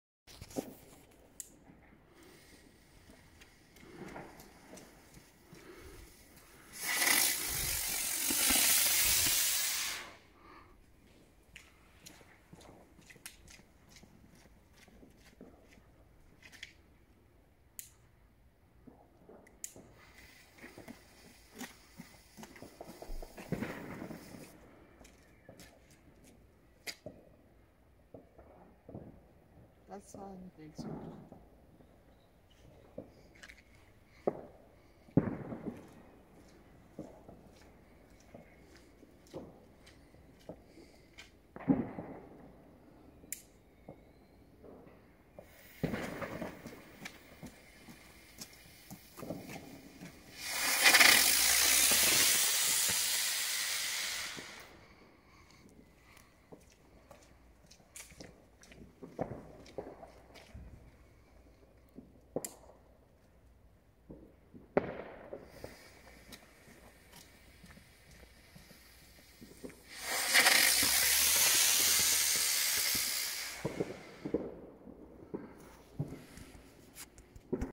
Night Crawlers von Lesli
Geschossen an Silvester 2020/2021 auf Privatgrund, es handelt sich hier um legales Silvesterfeuerwerk der Kat. F2

Bewertung: 8 von 10 Punkten

Ein Set aus zwei bekannten Feuervögel, aber die sind echt besser geworden. Gute Steighöhe und gerader Aufstieg, leider sind zwei nicht gestartet (Aus dem ganzen Set)